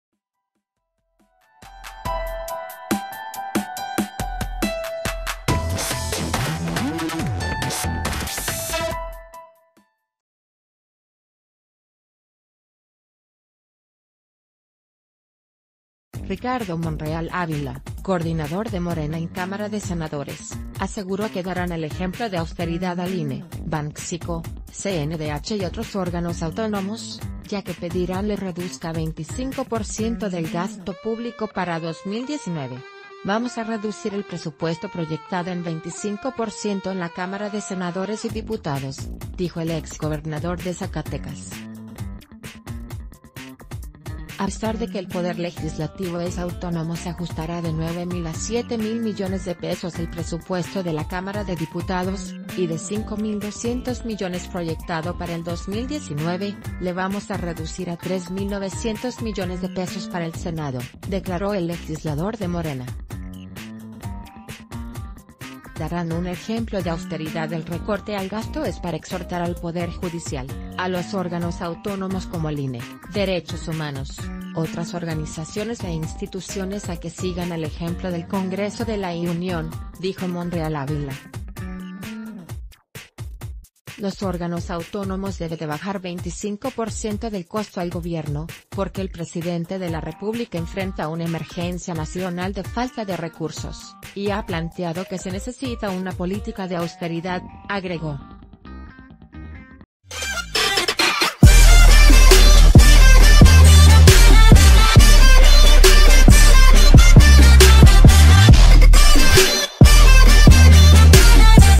Reducirán 25% presupuesto en Cámara de Diputados y Senadores para 2019
Ricardo Monreal Ávila, coordinador de Morena en Cámara de Senadores, aseguró que darán el ejemplo de austeridad al INE, Banxico, CNDH y otros órganos autónomos, ya que pedirán les reduzca 25 por ciento del gasto público para 2019.“Vamos a reducir el presupuesto proyectado en 25 por ciento en la Cáma...